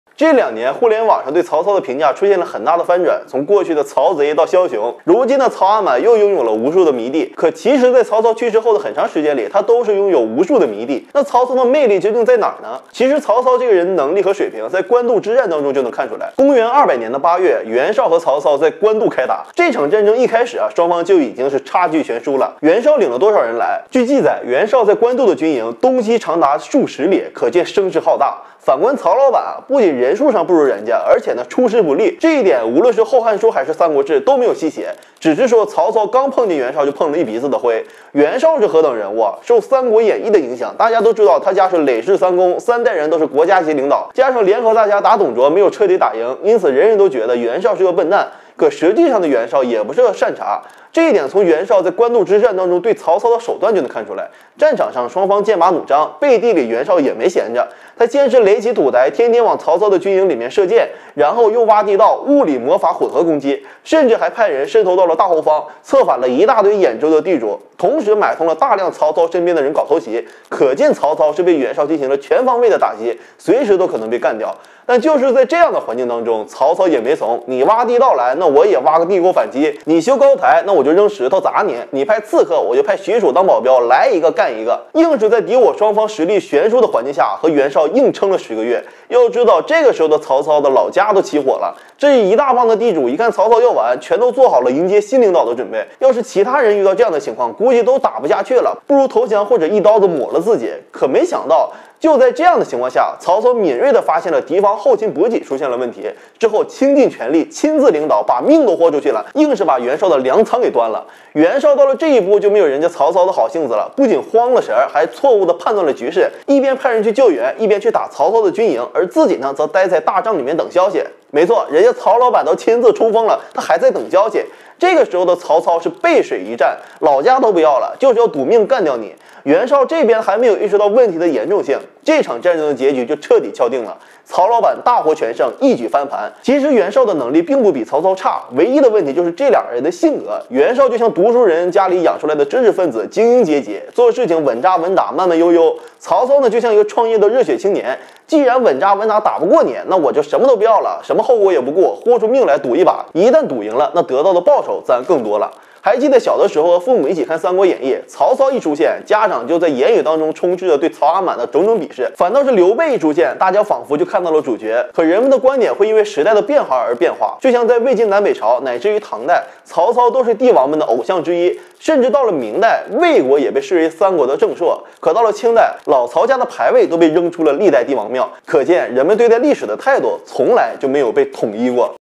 官渡之战袁绍占有绝对优势，为啥却一败涂地？曹操的军事能力被历史低估了！
官渡之战是汉末三国三大战役之一，它为曹操后来统一北方奠定了基础。这场战争曹操能获取胜利除了和他非凡的军事才能有关以外，还有一些偶然事件的发生决定了官渡之战的结局。事实上曹操在官渡之战前期一直出于劣势，后来袁绍的一大致命弱点被曹操发现后，局势才发生了逆转。